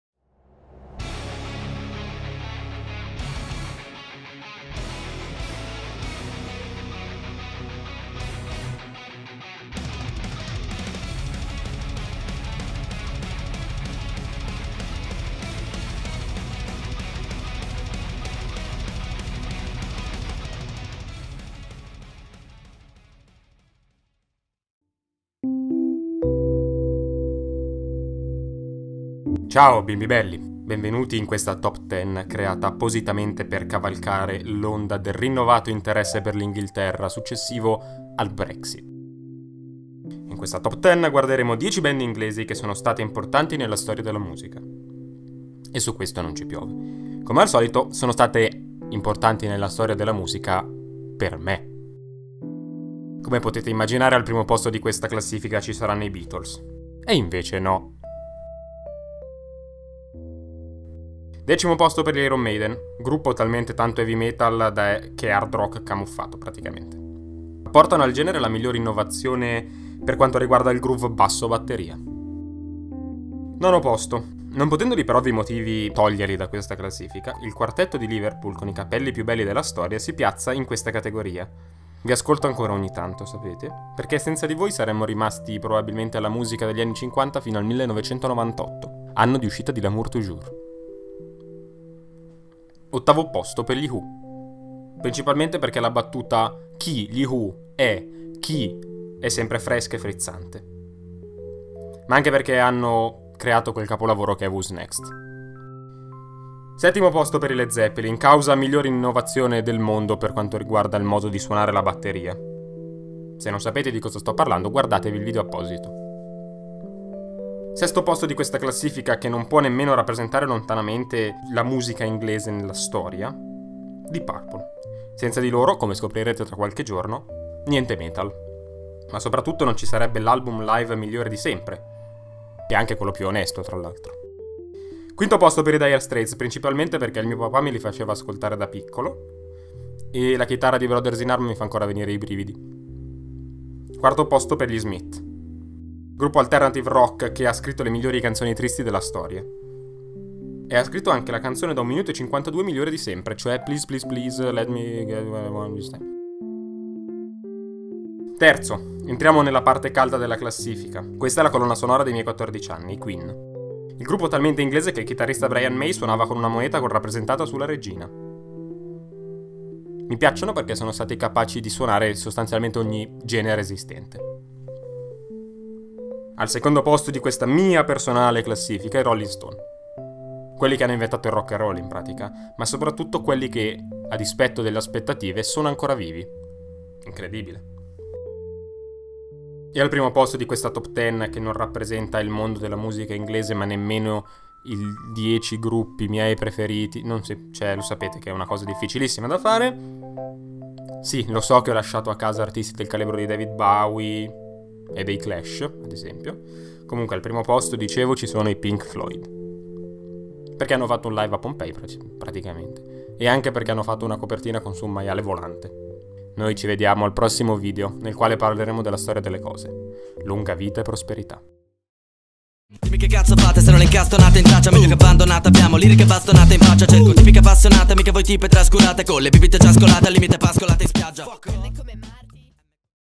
Top 10 Brexit - I 10 artisti inglesi più importanti di sempre
Un piccolo video che cavalca l'onda del brexit ma si schianta sugli scogli della mia incapacità di essere simpatico.
Qual è la vostra top 10 delle migliori band/artisti inglesi?